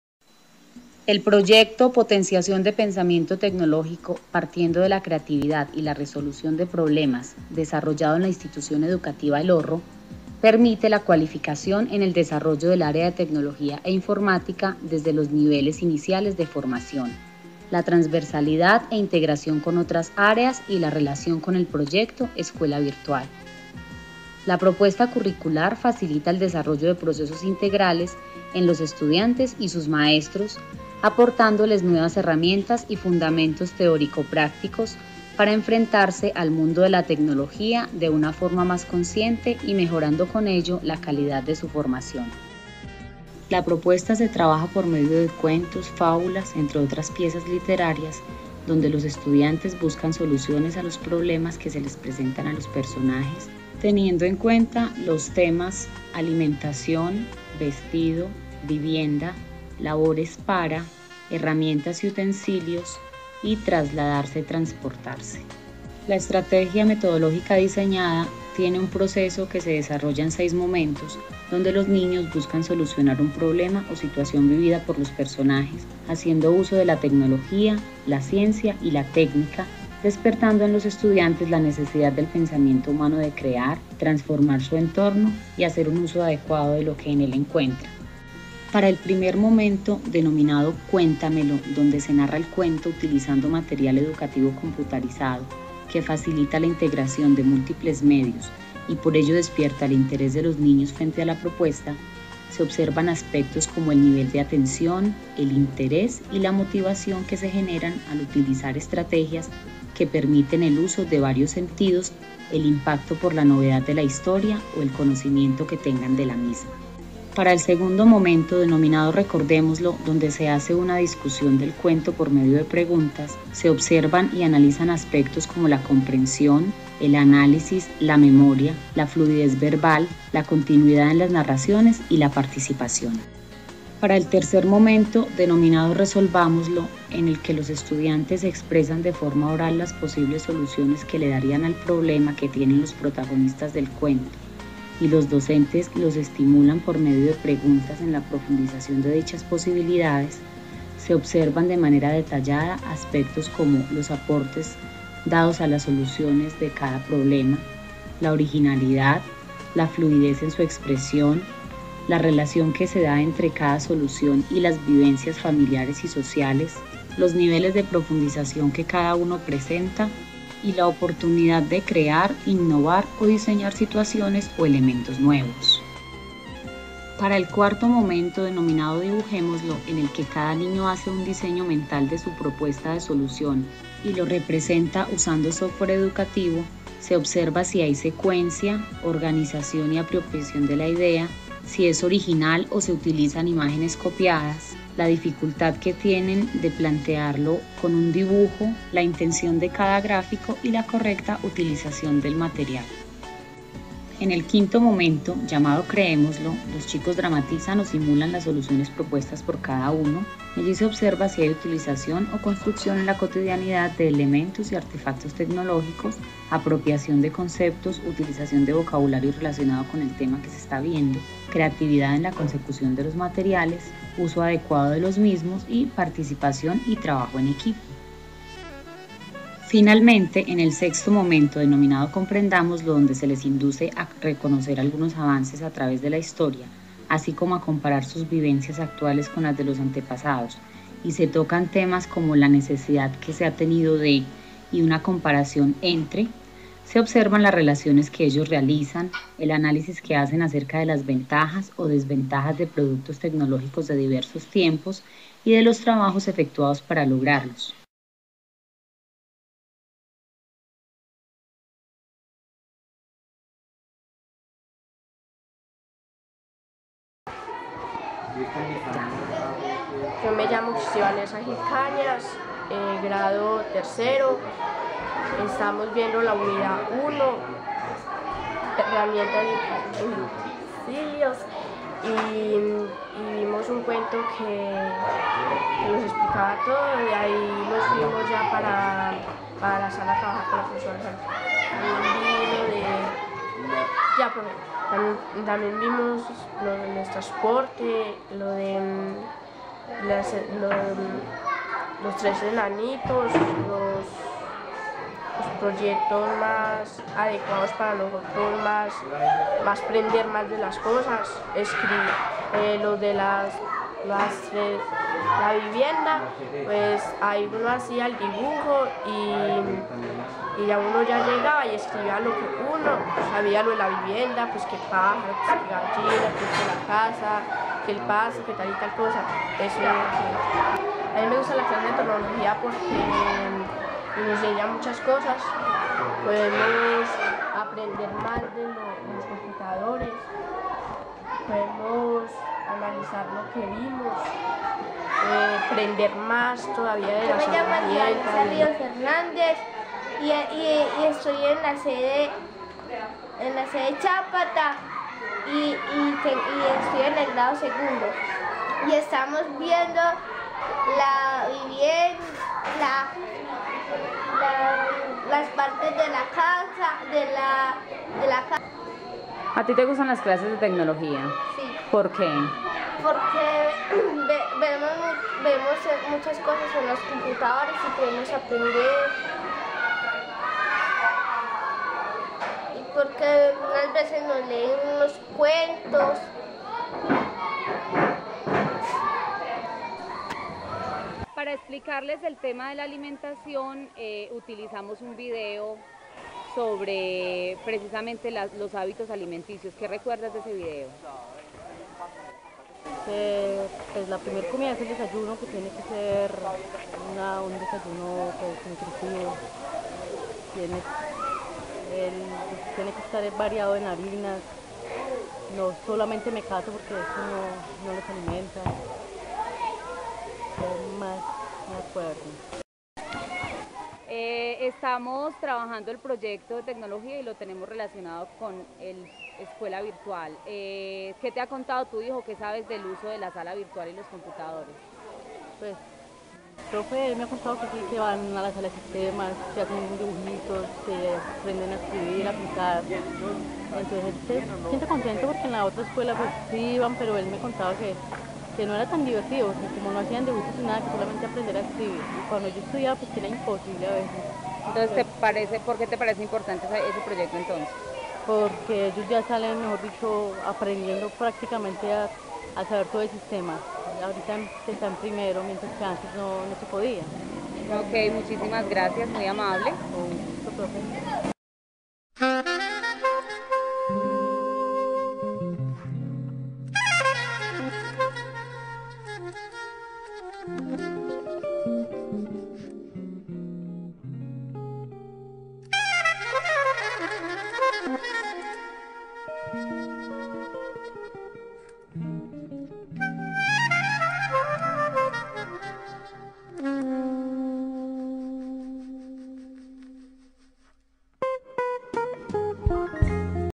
POTENCIACIÓN DE PENSAMIENTO TECNOLÓGICO - INSTITUCIÓN EDUCATIVA EL HORRO - ANSERMA CALDAS
Proyecto desarrollado con la comunidad educativa de la Institución Educativa El Horro de Anserma Caldas - Colombia.
Nace en 2005, como un proyecto de investigación dentro del área de Tecnología e Informática, en el marco de formación de la docente Sandra Milena Carvajal Alarcón, cuando cursaba Licenciatura en Tecnología e Informática en la Universidad Católica de Manizales.
El proyecto continúa, y es acogido por la I. E. El Horro como programa de formación para los estudiantes de los grados preescolar a tercero y se articulan sus componentes pedagógicos y los principios de formación del área al PEI de la institución. Algunas docentes de la institución han participado en la revisión del proyecto.
En el proceso también se ha capacitado a los docentes de la institución en la implementación del proyecto, la secuencia didáctica y sus componentes, así como se ha asesorado a algunas sedes en aspectos del programa Escuela Virtual del área de educación del comité de Cafeteros de Caldas.